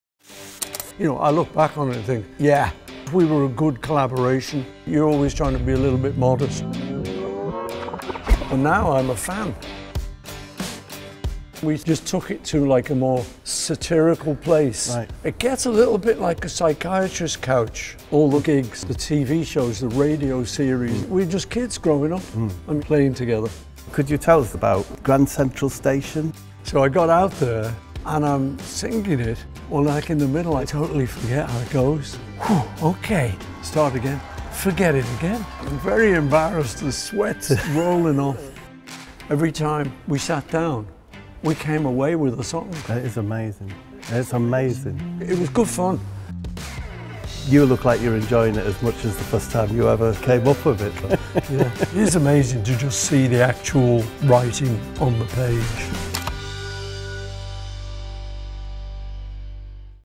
THE LYRICS: 1956 to the Present (Teaser Trailer #2)
In a teaser trailer for his new book THE LYRICS, Paul talks to Bob Mortimer about looking back at The Beatles, his performance at Grand Central Station and much more.

Paul and Bob chatted at the British Library which will soon be hosting the new display 'Paul McCartney: The Lyrics' from 5th November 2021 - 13th March 2022.

THE LYRICS, publishing next week on 2 November, will tell the story of Paul McCartney’s life and art through the prism of 154 songs from all stages of his career.

THE LYRICS: 1956 to the Present
by Paul McCartney

Edited and Introduced by Paul Muldoon
Publishing 2nd November, 2021